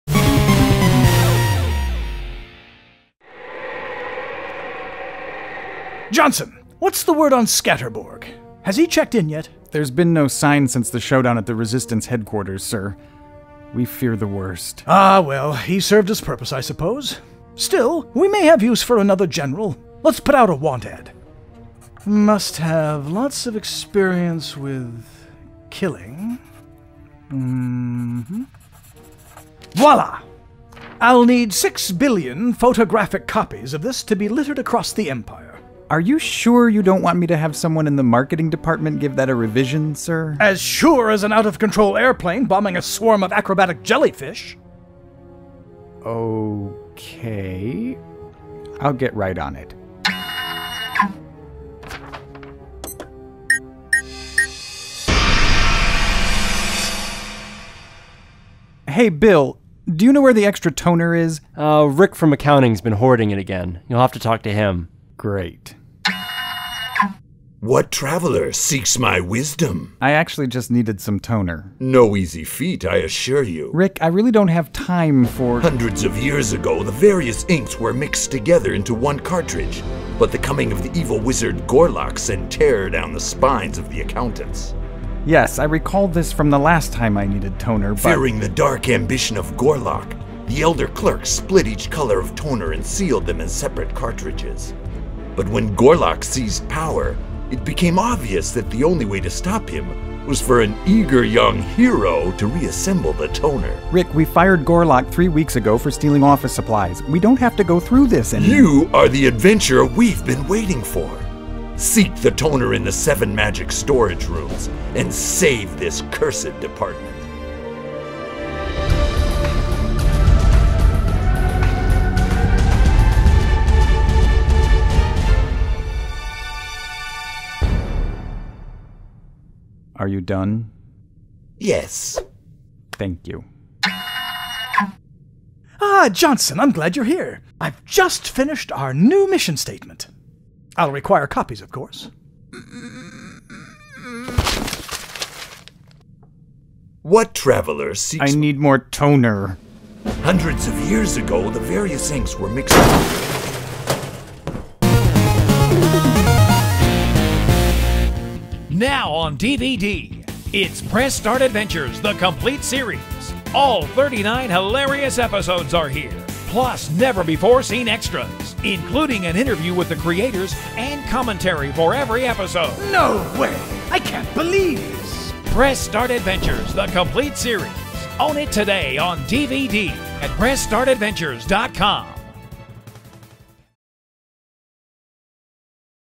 Press Start Adventures: F-Xerox
Johnson's photocopying errand turns out to be more than he bargained for. Featuring the voice of Castlevania: Symphony of the Night's Alucard, Robert Belgrade!